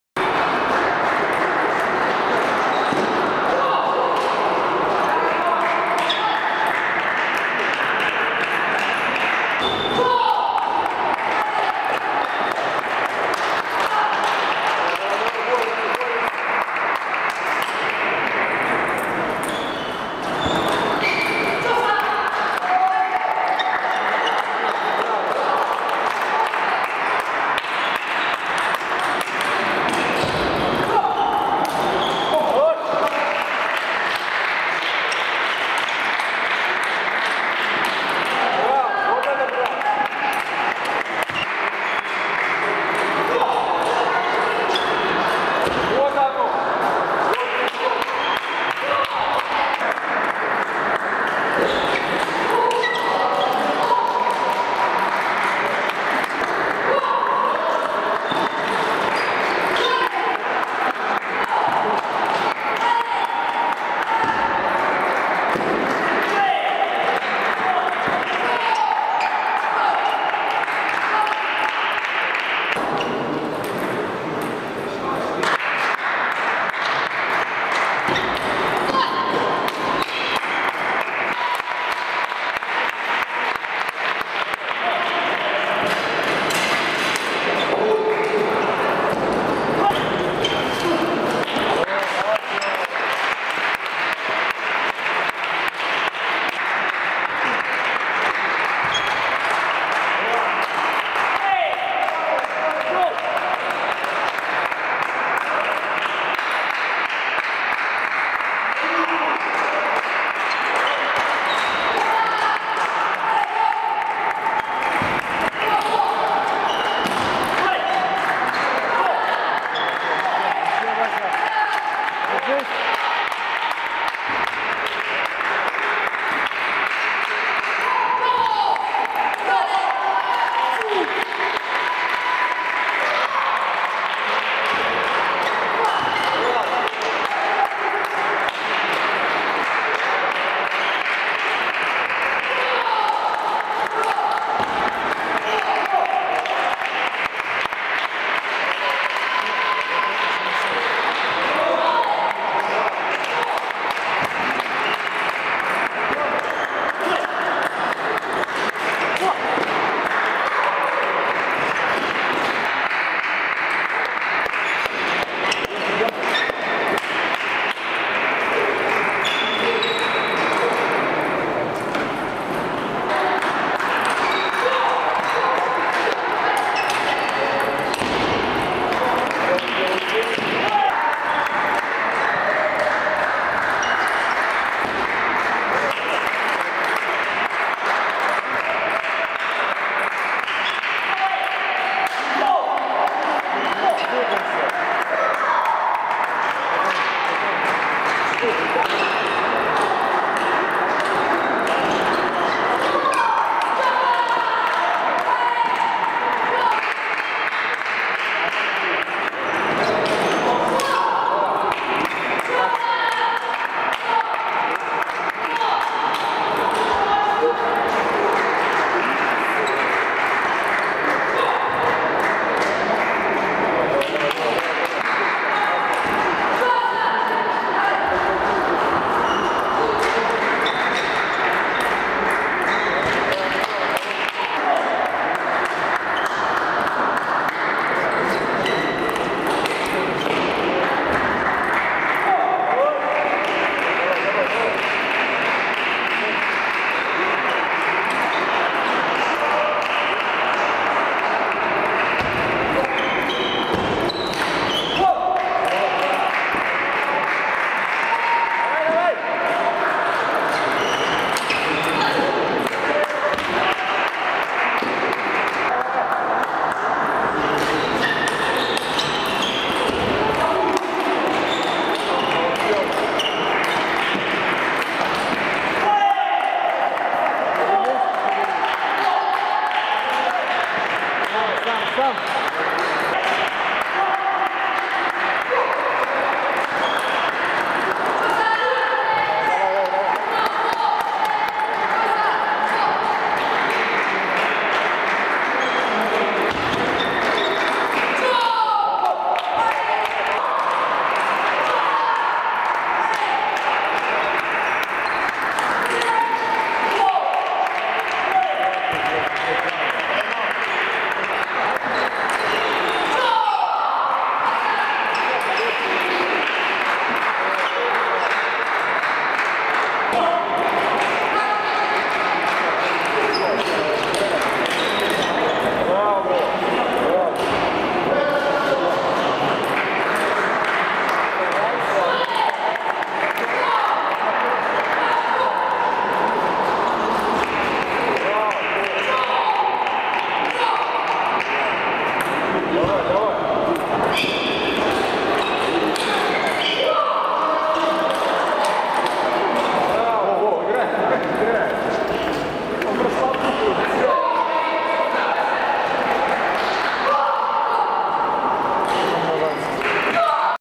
ТОП -24 2014 по настольному теннису, Зиронова - Семенова
Всероссийский турнир сильнейших спортсменов России по настольному теннису ТОП-24 среди юношей и девушек до 16-ти и 19-ти лет. Архангельск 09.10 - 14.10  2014. Первый финал среди кадетов, Екатерина Зиронова - Дарья Семенова.
Russian table tennis youth championship TOP-24 , Zironova - Semenova.